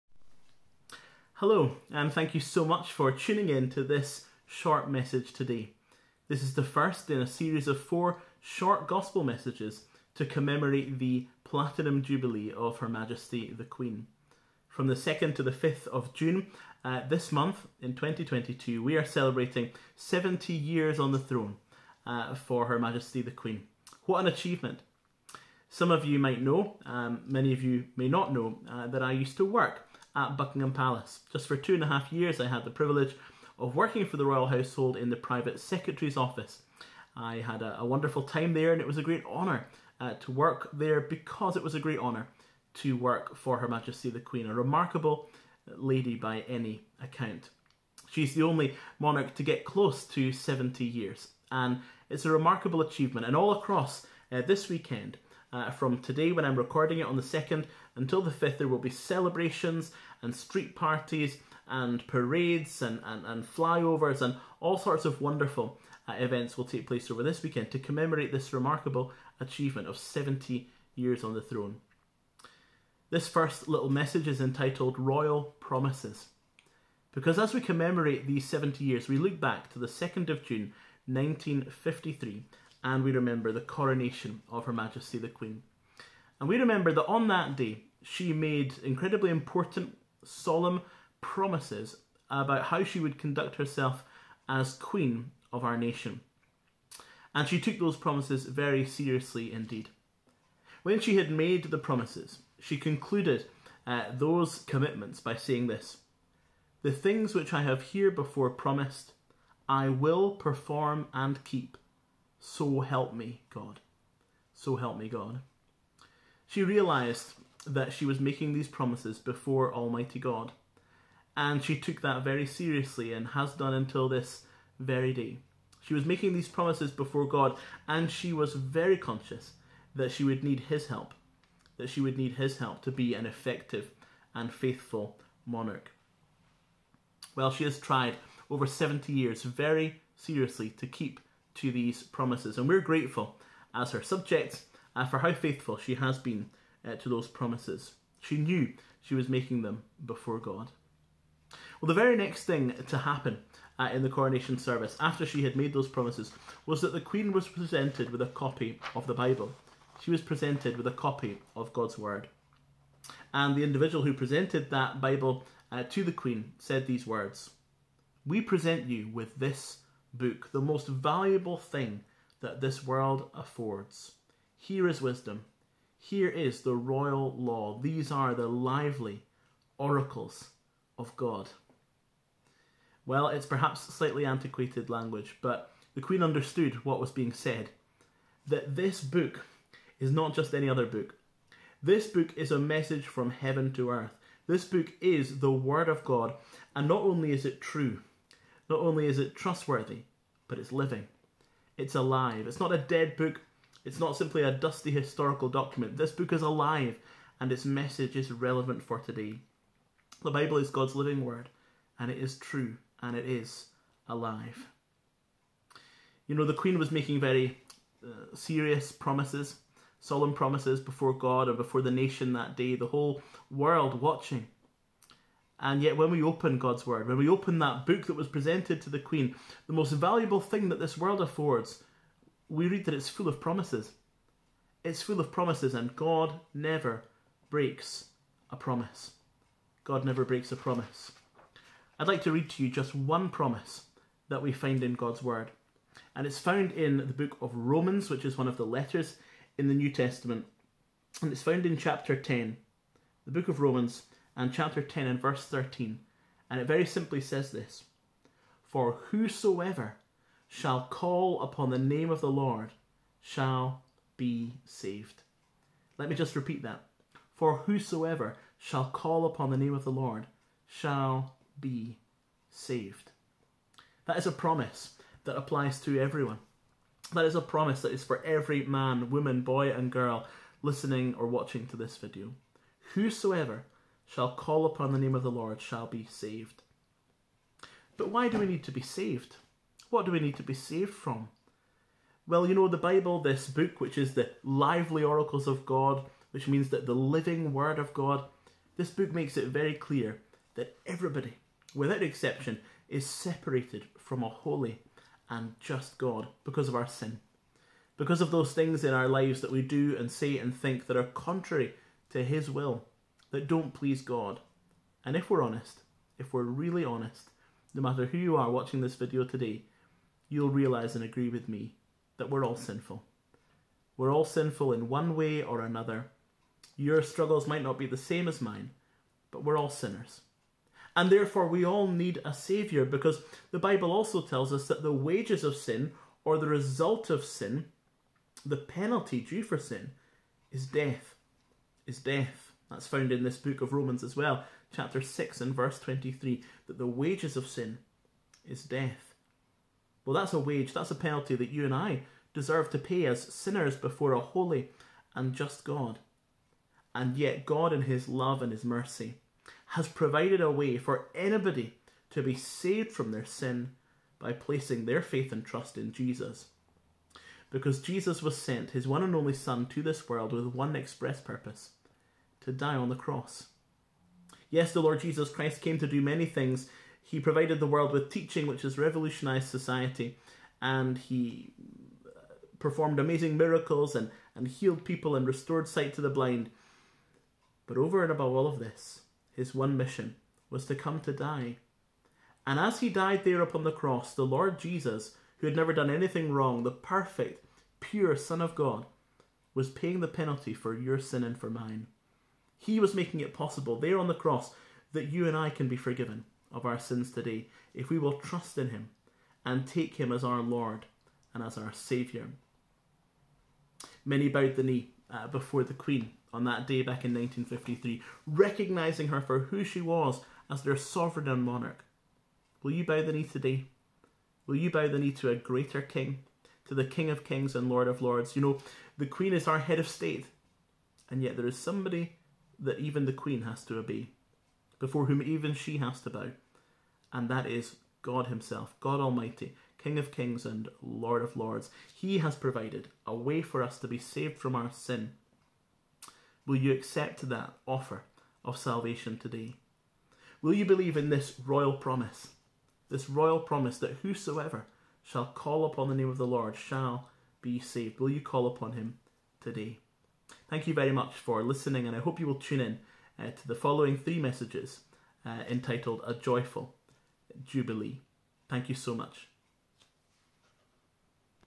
'Joyful Jubilee' 1. Royal Promises
A brief Gospel message prepared for the believers at Holburn Gospel Hall, Aberdeen.